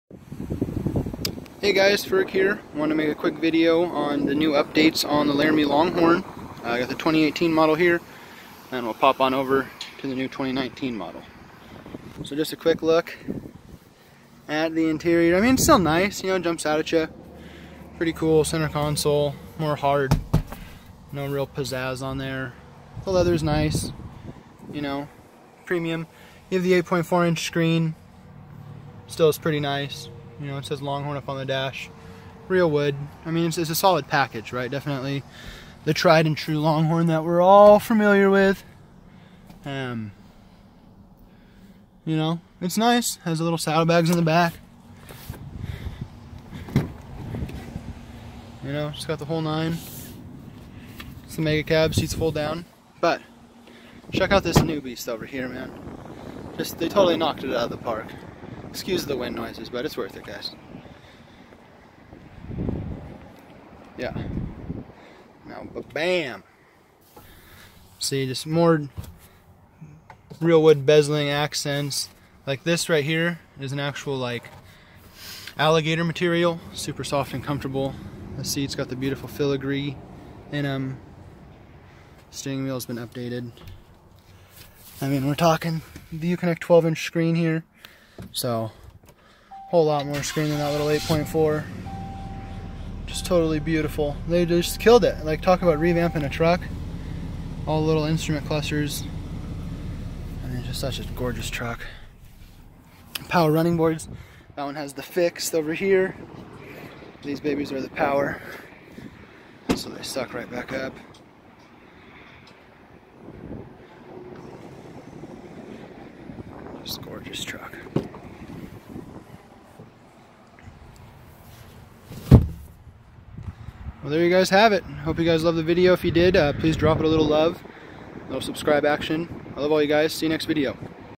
2019 Ram HD longhorn VS 2018 Longhorn. Wow the upgrades!
Its really hard to beat the new 2019!